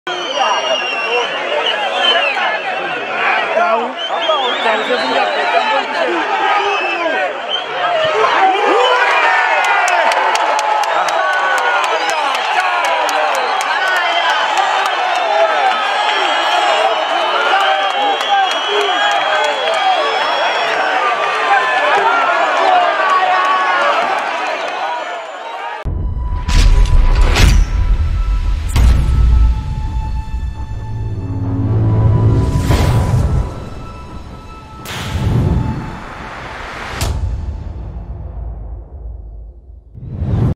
Everything wrong about defending is in this video
watch how Dynamos conceded the second goal against Bulawayo Chiefs